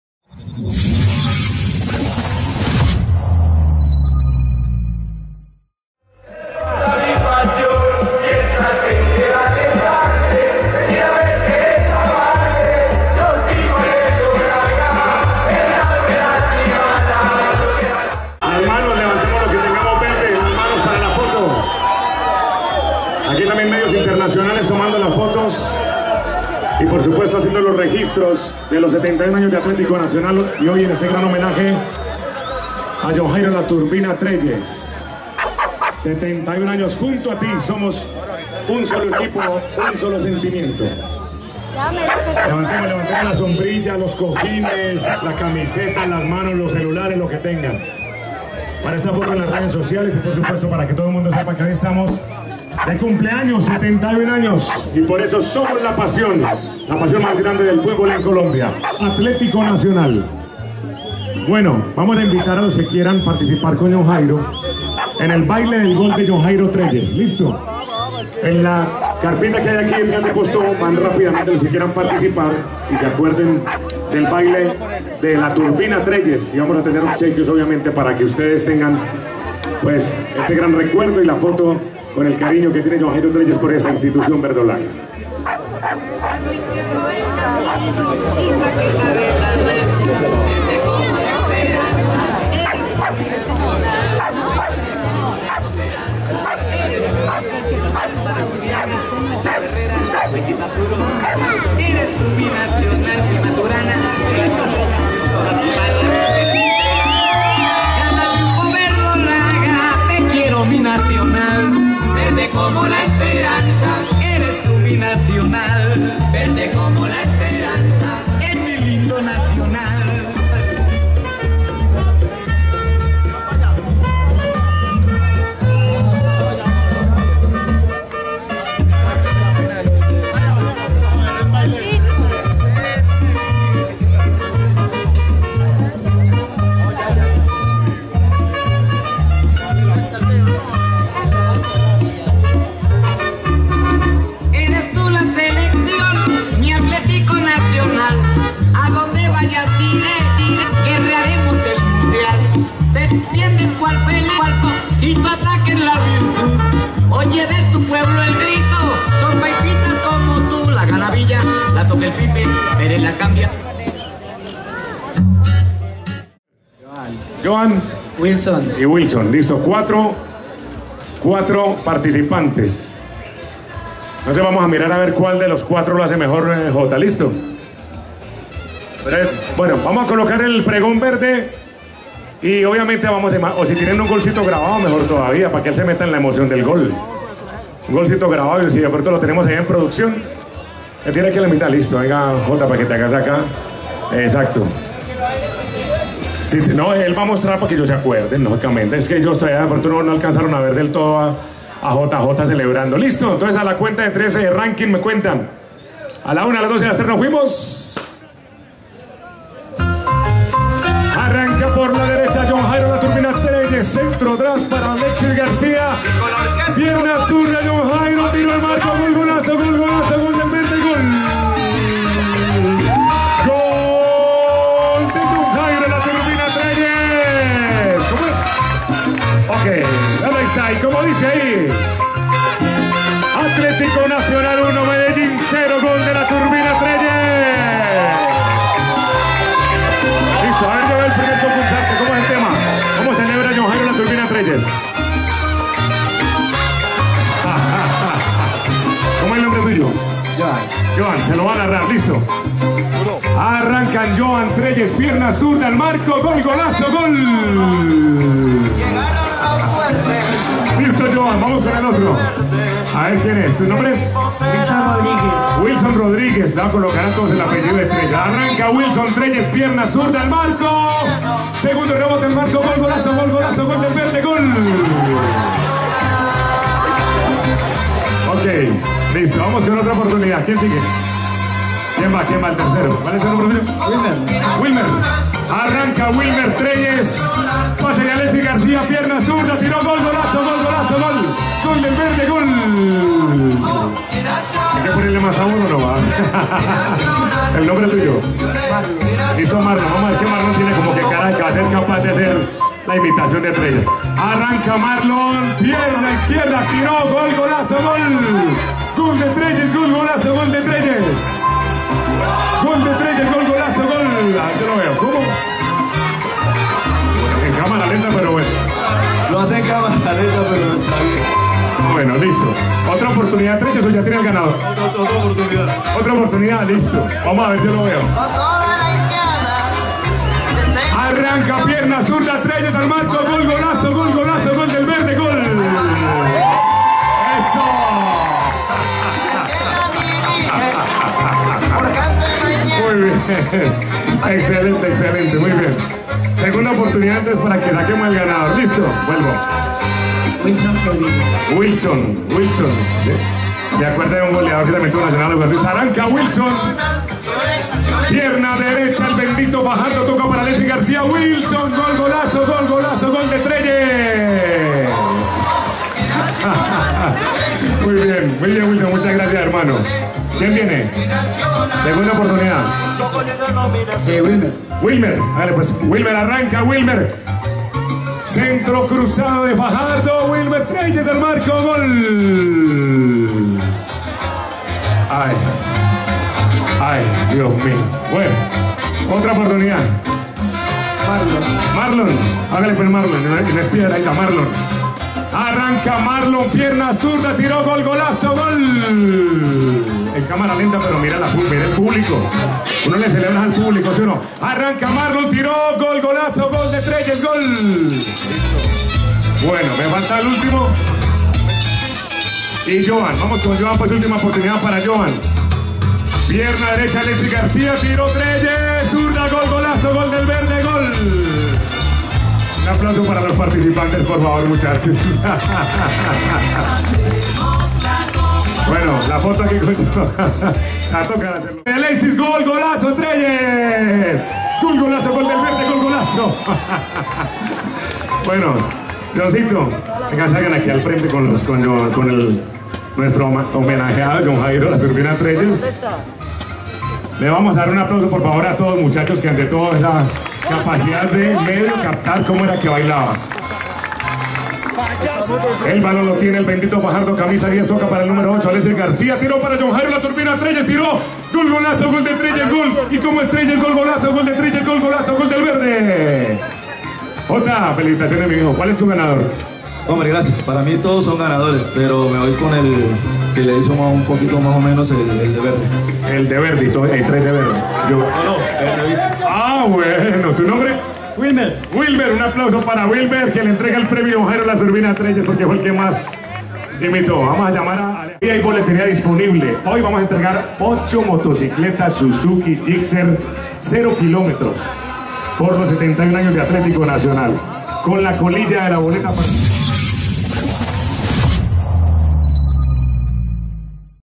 Atlético Nacional:"Baila como Trellez, en la Celebración de los 71 años"...
Atlético Nacional cumplió 71 años de existencia en el fútbol profesional Colombiano y como parte de la celebración, le hicieron un merecido homenaje a Jhon Jairo"La Turbina" Trellez,, un jugador que nos regaló muchos triunfos y nos puso a vibrar, a gozar y a bailar con sus goles.

Además Nacional, realizó un concurso en parque de banderas, algo así como un "BAILA COMO TRELLEZ", varios hinchas verdolagas se apuntaron para tratar de imitar el famoso baile de la turbina y tratar de ganar el premio establecido.

NEP, estuvo ahí, los grabó bailando y se los presentamos para que todos gocemos un poco con estos valientes hinchas que se atrevieron a bailar como TRELLEZ...
Veamos...